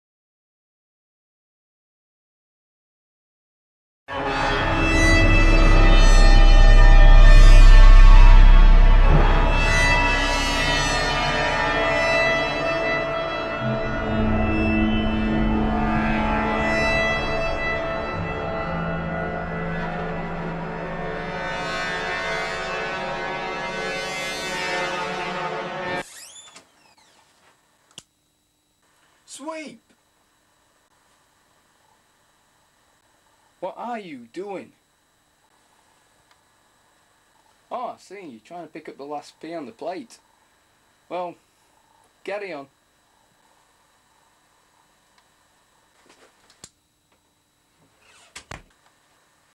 sweep masochist?
this is a short film i made yesterday! i got the sweep puppet when i was eight years old at a sooty & co museum i went to! its very short and admittedly not too much time was spent on making it but after a few tweaks here and there i think it will approve lol the real question is is sweep a masochist? enjoy

hounors for this video

#28 - Top Favourites (Today) - Film & Animation
#22 - Top Rated (Today) - Film & Animation
#51 - Most Discussed (This Week) - Film & Animation